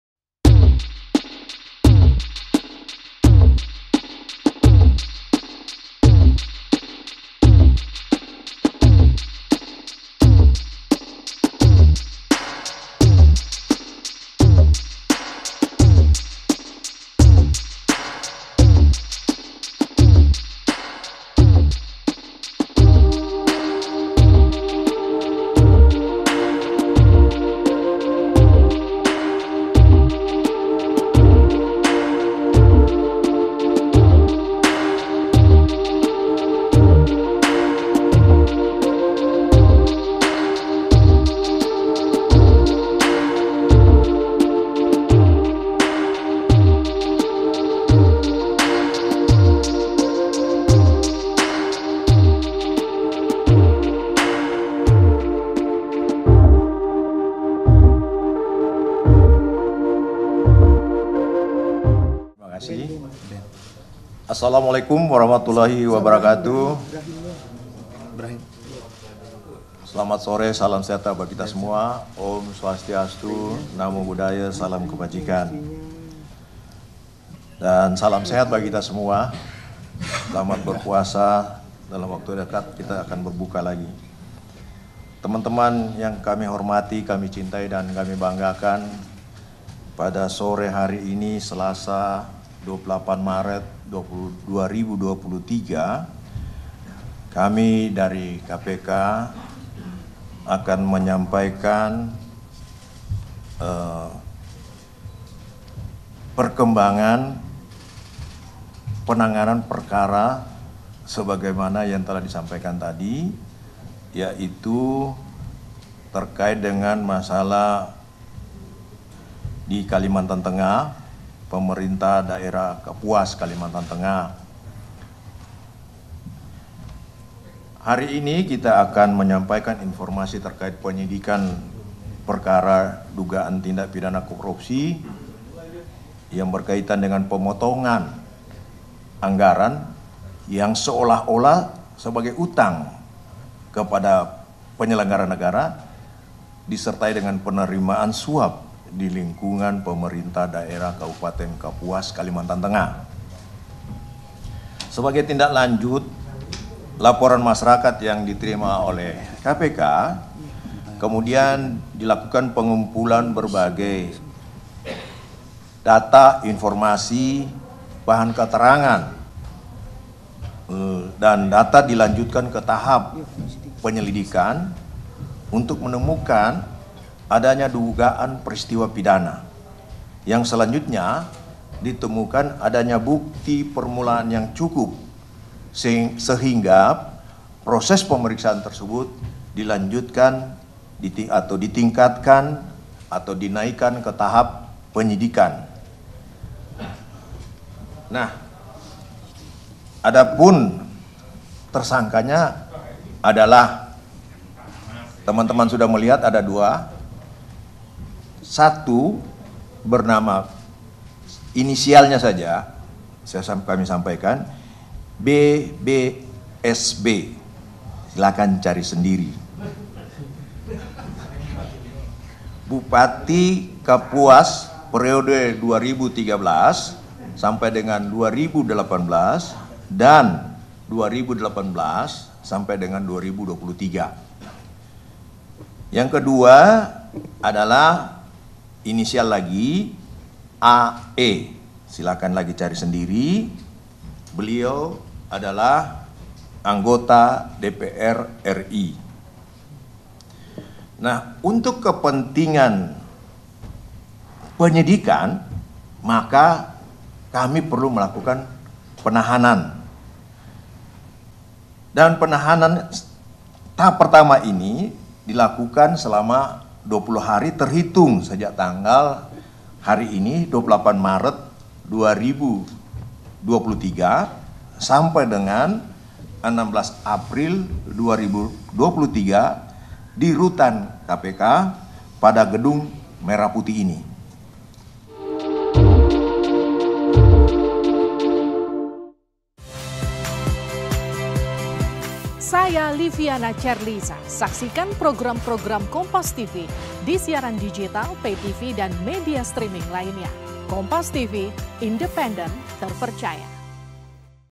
KPK Tahan Bupati Kapuas dan Istri Selama 20 Hari Usai Ditetapkan Sebagai Tersangka Korupsi
JAKARTA, KOMPAS.TV - Komisi Pemberantasan Korupsi (KPK) menahan Bupati Kapuas Ben Brahim S Bahat dan istrinya yang merupakan seorang anggota DPR RI, Ary Egahni, pada Selasa (28/3/2023).

Upaya ini dilakukan setelah keduanya ditetapkan sebagai tersangka kasus dugaan korupsi pemotongan anggaran di Pemerintah Kabupaten Kapuas, Kalimantan Tengah.

"Untuk kepentingan penyelidikan maka kami melakukan penahanan selama 20 hari terhitung hari ini 28 Maret sampai dengan 16 April 2023 di Rutan KPK pada Gedung Merah Putih," kata Wakil Ketua KPK, Johanis Tanak.

Ben selaku Bupati Kapuas menerima fasilitas dan uang dari Satuan Kerja Perangkat Daerah (SKPD) di Pemerintah Kabupaten Kapuas.

Dia juga diduga menerima fasilitas dan uang dari pihak swasta.